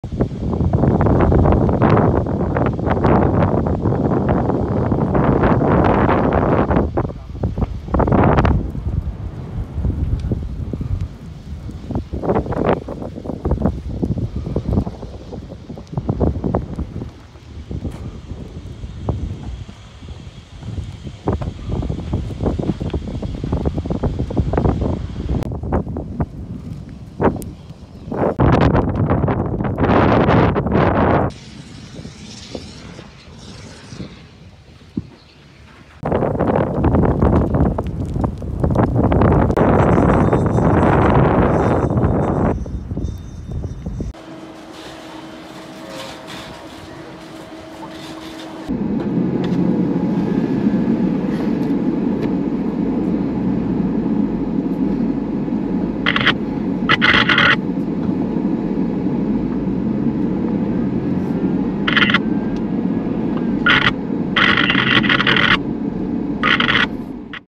Fire tears through Northern California town
A wind-driven wildfire tore through a Northern California mountain town, leaving much of downtown Greenville in ashes. (Aug. 5)